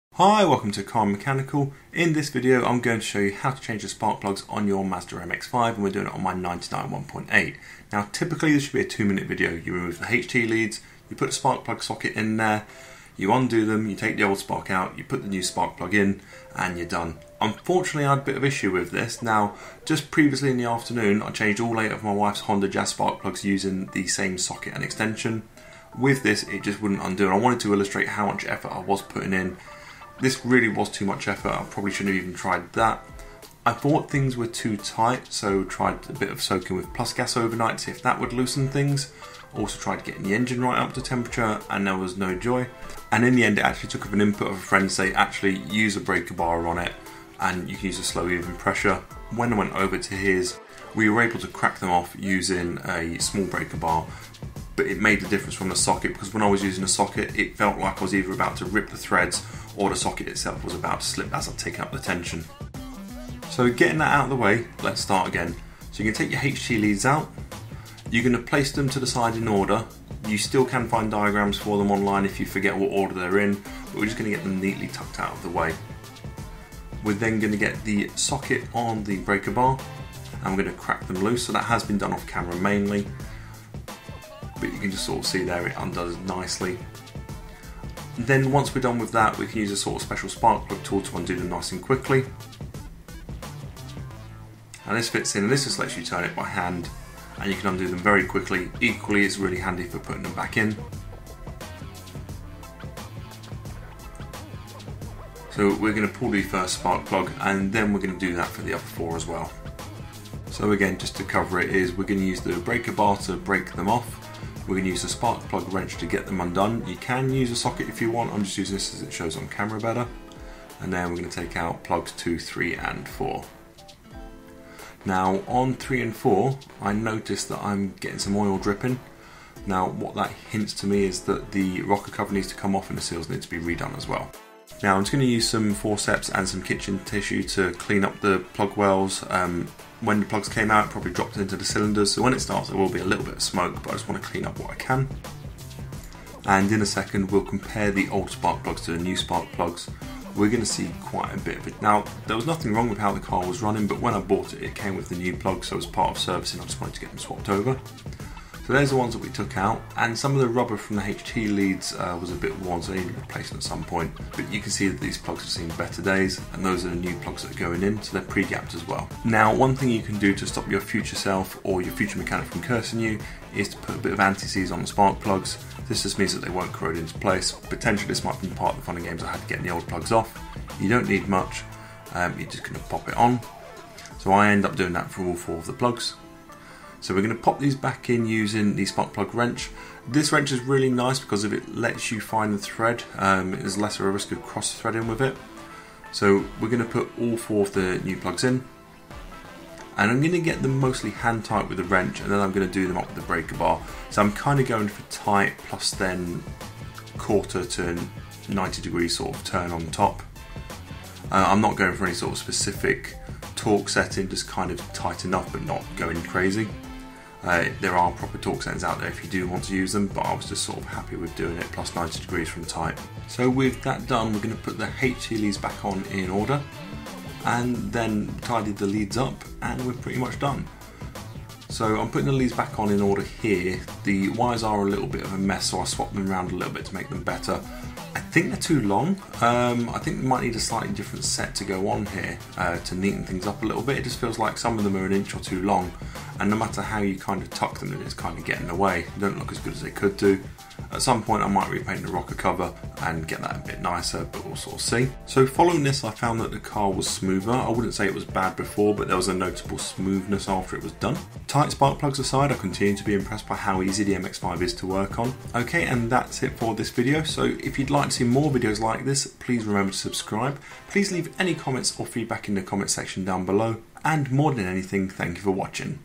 How to change Mazda MX5/Miata stuborn spark plugs
In this video I will show you how to change your Mazda MX5/Miata spark plugs, including what to do when they are stuborn!